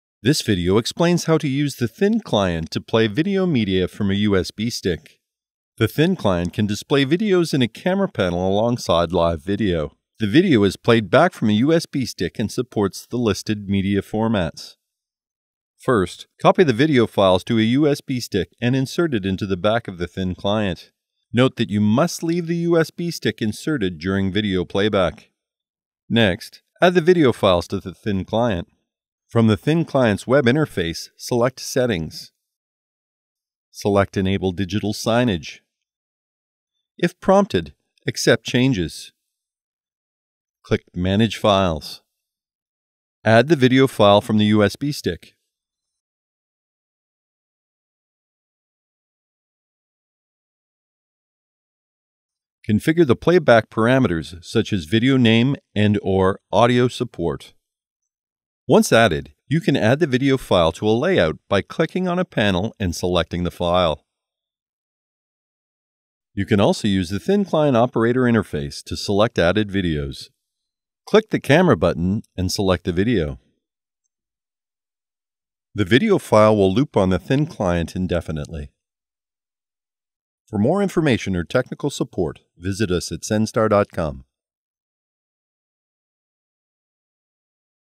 Playing Video Files on the Thin Client
This video explains how to use the Senstar Thin Client to play video media from a USB stick.